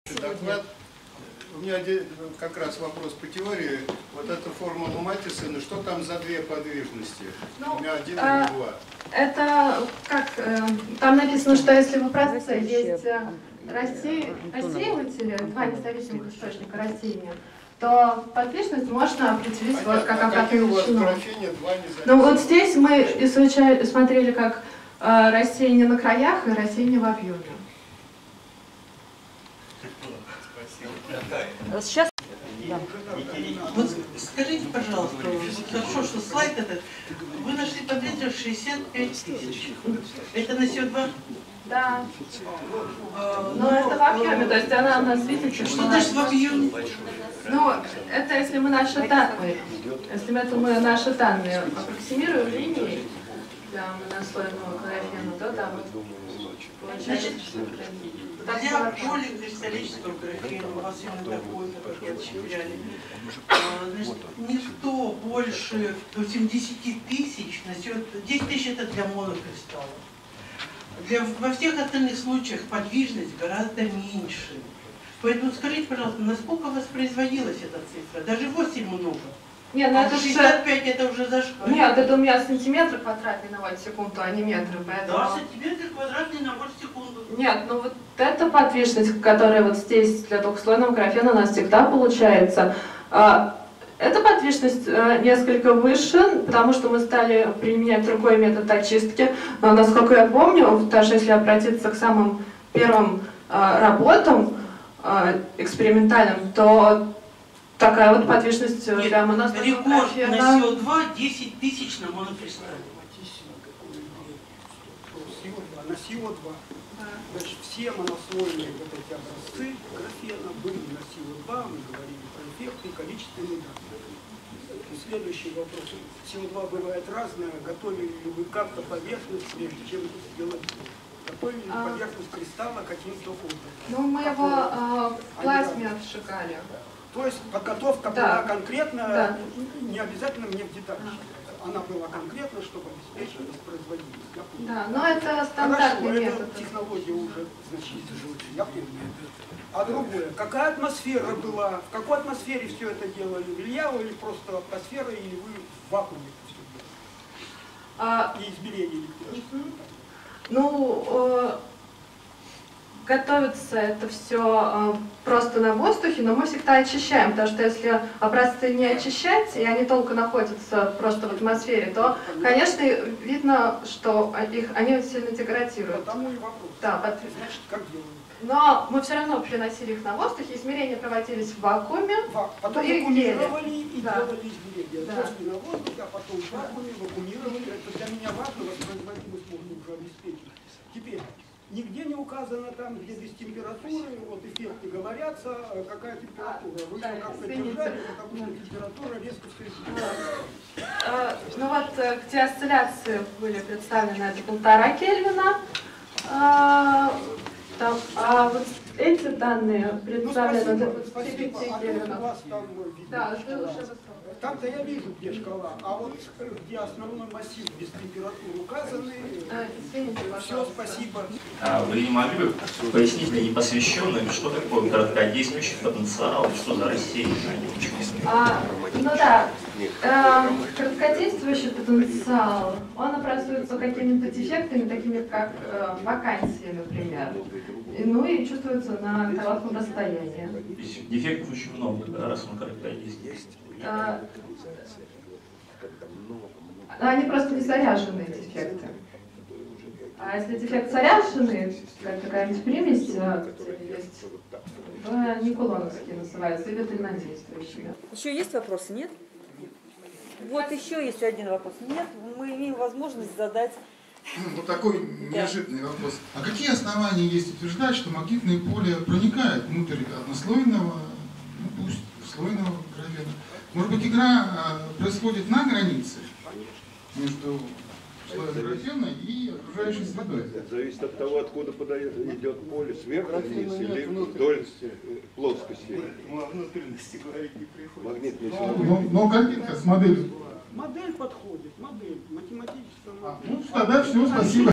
046. Васильева Г.Ю. дискуссия (2/2)
Васильева Г.Ю. дискуссия: МАГНЕТОСОПРОТИВЛЕНИЕ ГРАФЕНА: ГЕОМЕТРИЧЕСКИЙ ФАКТОР И МЕХАНИЗМЫ РАССЕЯНИЯ